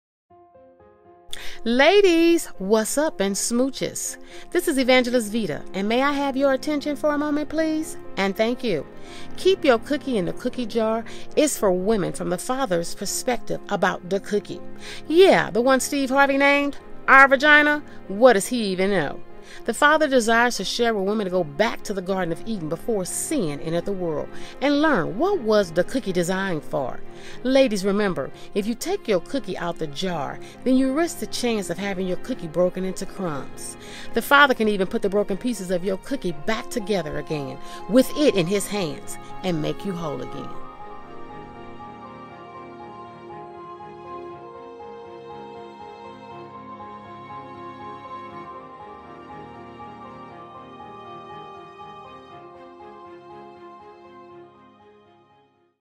Keep Yo Cookie in Da Cookie Jar Book Trailer
Ladies IT'S HERE! The Book Trailer for Keep Yo Cookie in Da Cookie Jar!

KEEP YO COOKIE IN DA COOKIE JAR AVAILABLE TOMORROW June 7, 2019